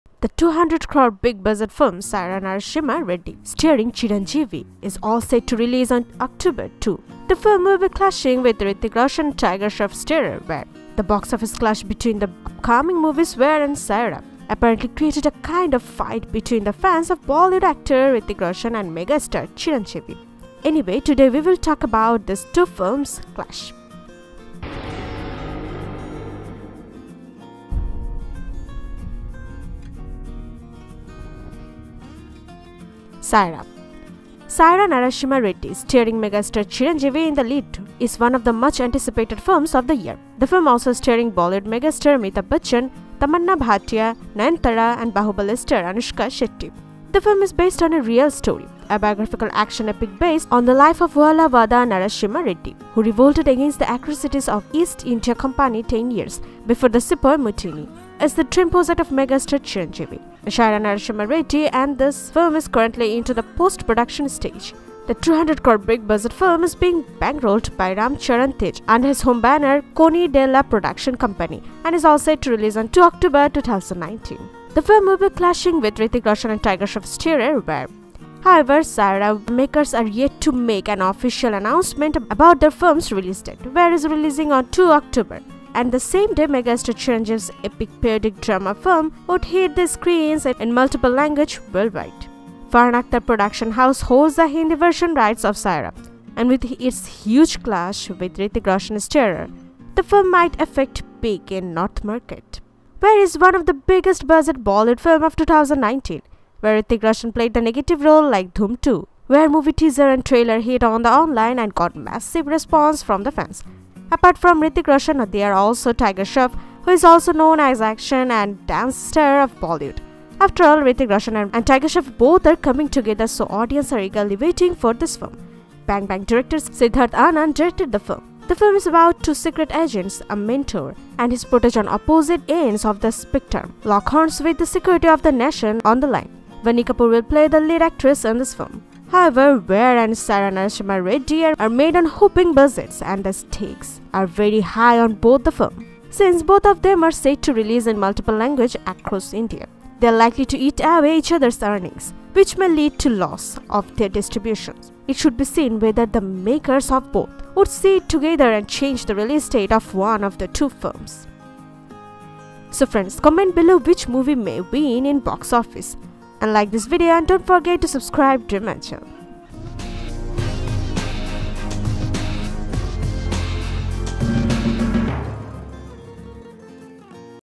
Box Office Clash | War and Sye Raa Narasimha Reddy Movie | Hrithik Roshan VS Chiranjeevi
Box Office Clash | War and Sye Raa Narasimha Reddy Movie | Hrithik Roshan VS Chiranjeevi. Clash in Bollywood is a Common matter. Recently Akshay Kumar Mission Mangal and John Abraham Batla House making clash in Box Office. Both Films making Good profit. Now Hrithik Roshan and Tiger Shroff Starrer Most awaited upcoming Big Budget Film Making Clash with Tollywood most awaited film Sye Raa Narasimha Reddy. Today we will talk about Box Office Clash | War and Sye Raa Narasimha Reddy Movie | Hrithik Roshan VS Chiranjeevi

War
War is one of the Biggest Budget Bollywood film of 2019 Where Hrithik Roshan Played the Negative role like Dhoom 2. War Movie Teaser and Trailer Hit on the online and Got massive response from the fans. Apart from Hrithik Roshan there also Tiger Shroff who is also Known as Action and Dance star of Bollywood. After all Hrithik Roshan and Tiger Shroff both are coming together so audience are eagerly waiting for this film. Bang Bang director Siddharth Anand Directed the Film. There are unbelievable action sequence of Hrithik Roshan and Tiger Shroff. War Movie Trailer loved by the audience but The film is set to release 02 October 2019. On that day Telugu most awaited upcoming film Sye Raa Narasimha Reddy also Hit in screen.

Sye Raa Narasimha Reddy
Sye Raa Narasimha Reddy is an upcoming most awaited Tollywood film. Sye Raa Narasimha Reddy starring megastar Chiranjeevi in the lead is one of the much-anticipated films of the year. The film also stars Bollywood megastar Amitabh Bachchan, Tamannaah Bhatia, Nayanthara and Baahubali star Anushka Shetty among others. The film is based on a Real story.  Tollywood superstar Ram Charan is produce the film. Sye Raa Narasimha Reddy is a Biographical film where Hindi right won by Farhan akhter.